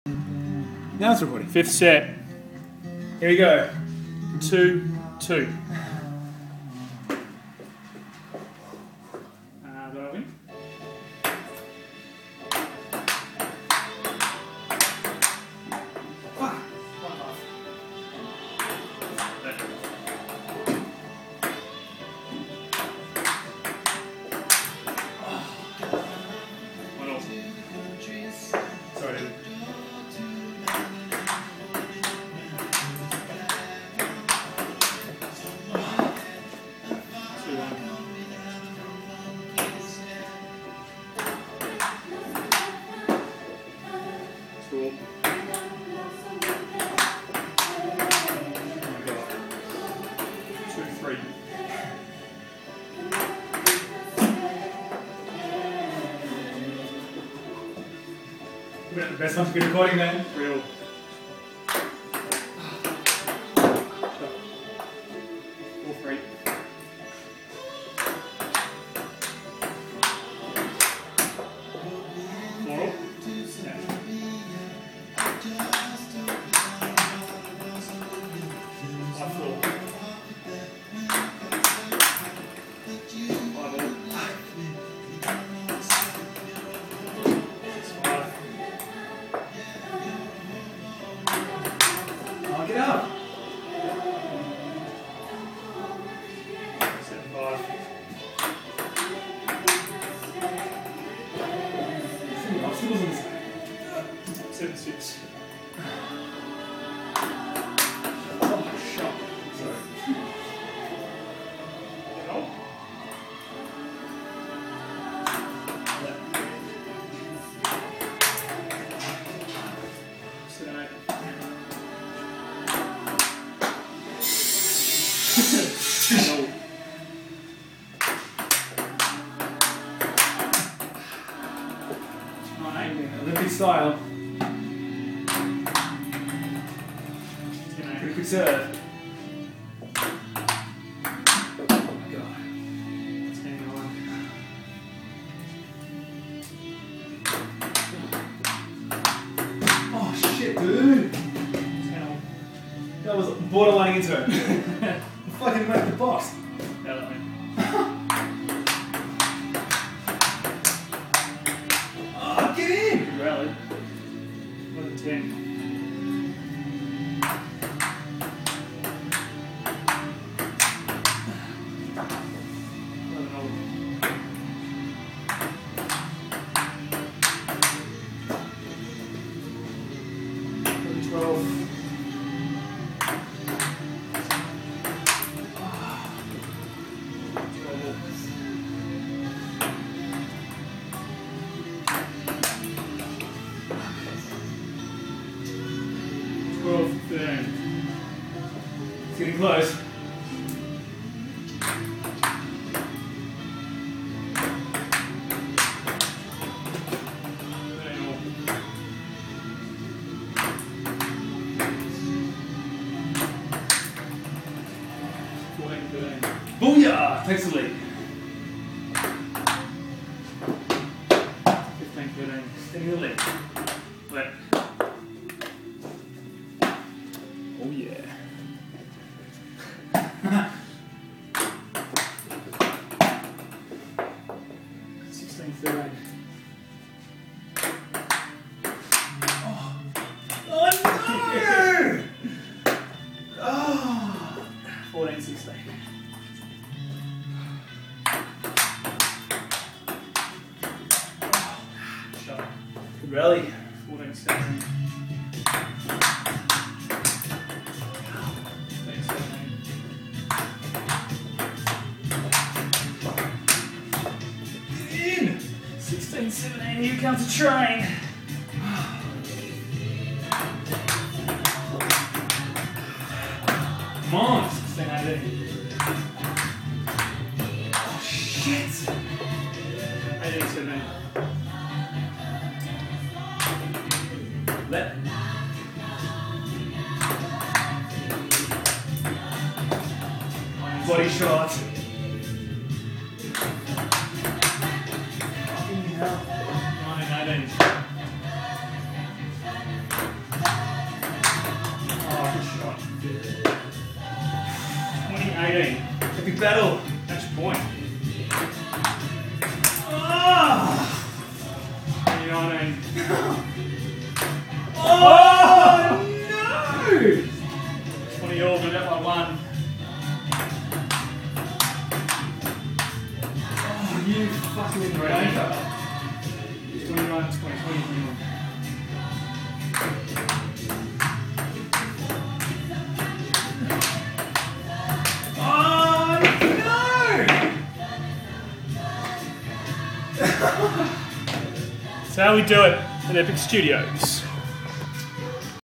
Lunch time table tennis battle
Chris from Epic Studios vs Callum 'The Beard' from Everything Travel Group